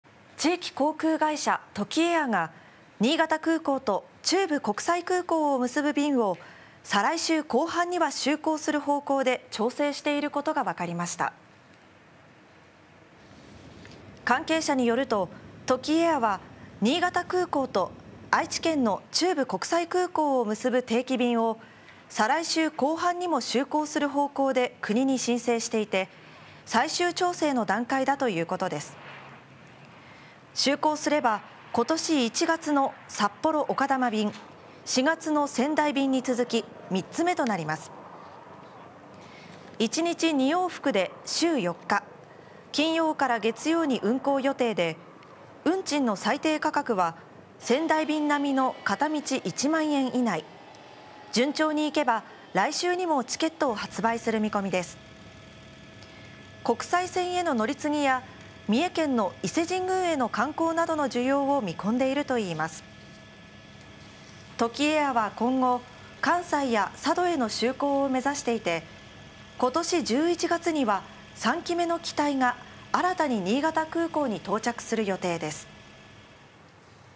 【トキエア】新潟と中部国際空港を結ぶ定期便 再来週後半に就航で調整 《新潟》
2024年9月10日放送

地域航空会社「トキエア」が新潟空港と中部国際空港を結ぶ便を再来週後半には就航する方向で調整していることがわかりました。

新潟県の最新ニュースや気になる話題、天気や防災情報、新型ウイルスなど最新情報をお伝えします。